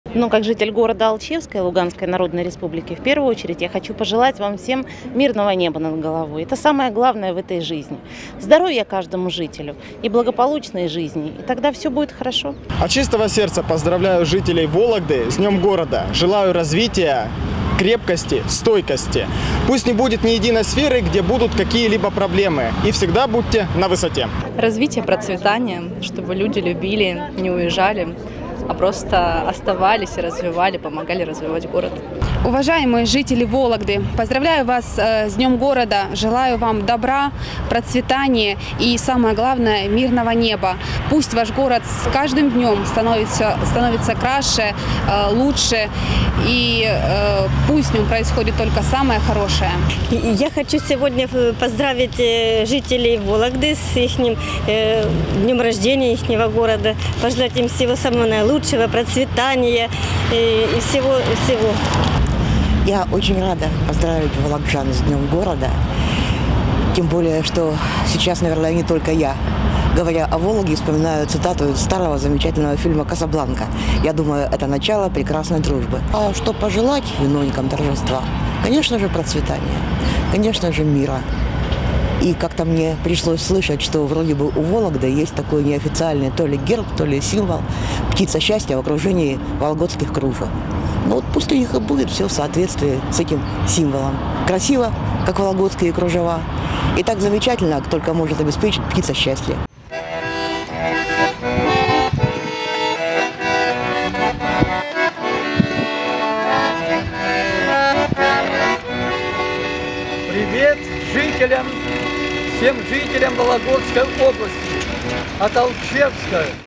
Жители Алчевска поздравили вологжан с Днём города
Вологда отмечает 875-летие. С этим событием вологжан поздравляют глава администрации Алчевска Луганской Народной Республики Альберт Апшев и его жители.

Напомним, что наш регион взял шефство над этим городом на Донбассе.

21 июня 2022 года между Вологодской областью и городом Алчевском было подписано соглашение о сотрудничестве.